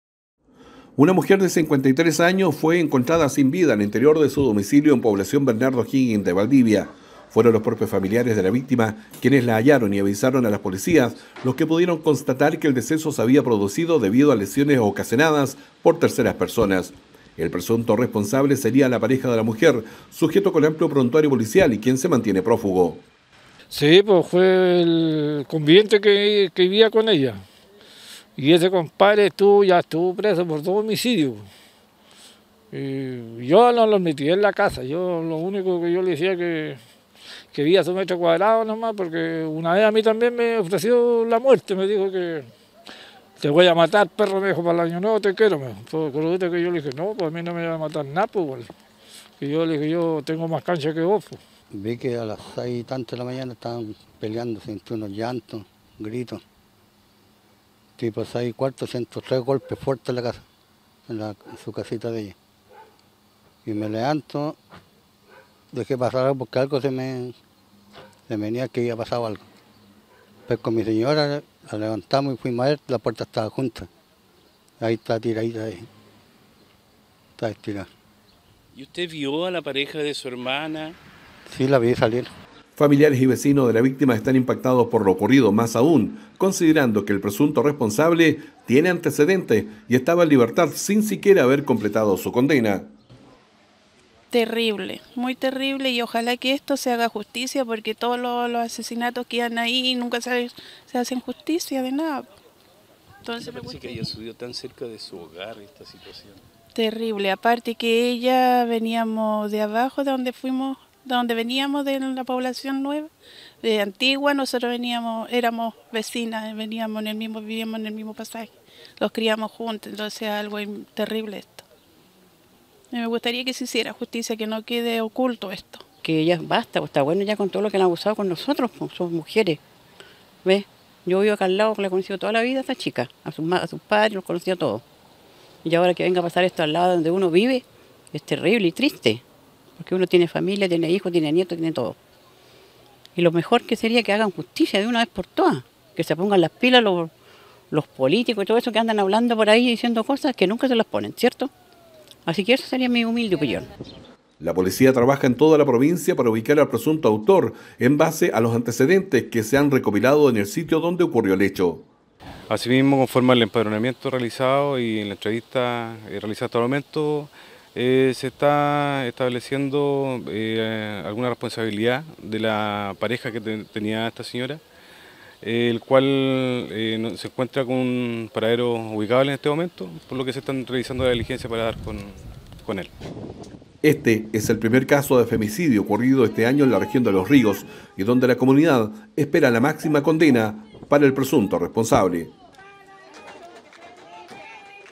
La policía busca intensamente a presunto autor de femicidio ocurrido en Valdivia
Una mujer de 53 años fue encontrada sin vida al interior de su domicilio en población Bernardo O”Higgins de Valdivia.